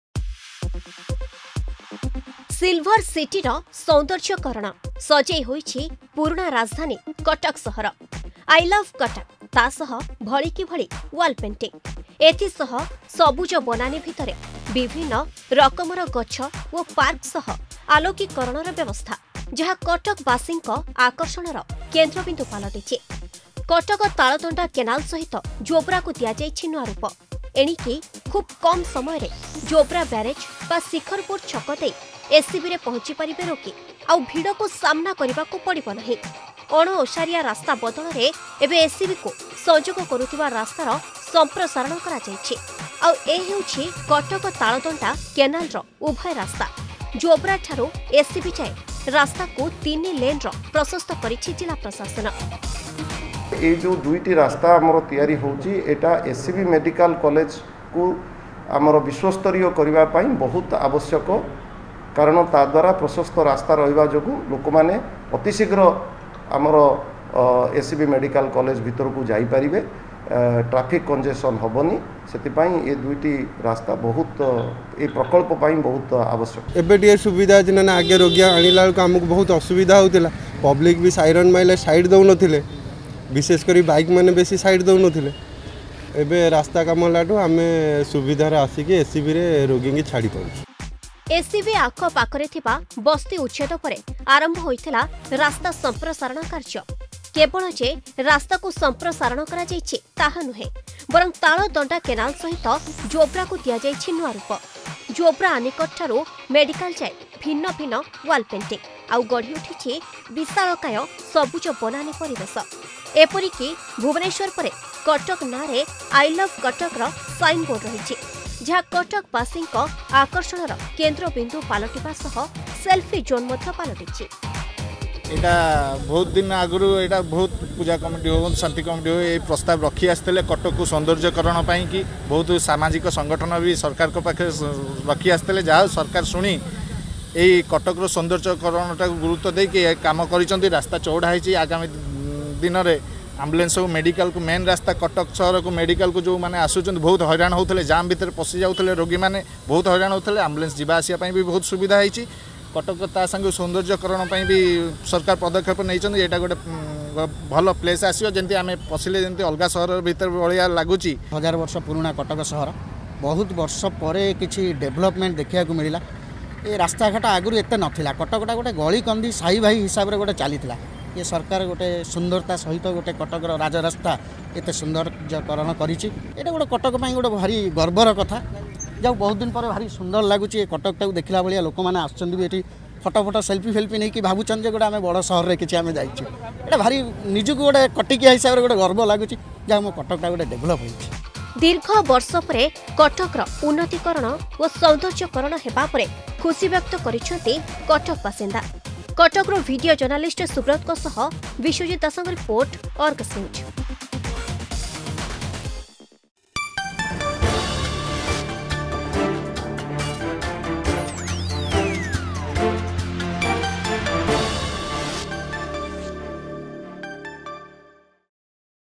ସିଲଭର ସିଟିର ସୌନ୍ଦର୍ଯ୍ୟକରଣ
ଦୀର୍ଘ ବର୍ଷ ପରେ ସୌନ୍ଦର୍ଯ୍ୟ କରଣ ହେଲା ପୁରୁଣା ରାଜଧାନୀ କଟକ ସହର । କଟକ ତାଳଦଣ୍ଡା କେନାଲ ସହିତ ଯୋବ୍ରାକୁ ଦିଆଯାଇଛି ନୂଆ ରୂପ । ଏଣିକି ଖୁବ କମ ସମୟରେ ଯୋବ୍ରା ବ୍ୟାରେଜ ବା ଶିଖରପୁର ଛକ ଦେଇ ଏସସିବିରେ ପହଞ୍ଚି ପାରିବେ ରୋଗୀ । ଏପରିକି ଭୁବନେଶ୍ୱର ପରେ ଏବେ କଟକ ନାଁରେ "ଆଇ ଲଭ୍ କଟକ"ର ସାଇନବୋର୍ଡ । ଯାହା କଟକ ବାସୀଙ୍କ ଆକର୍ଷଣର କେନ୍ଦ୍ରବିନ୍ଦୁ ପାଲଟିଛି ।...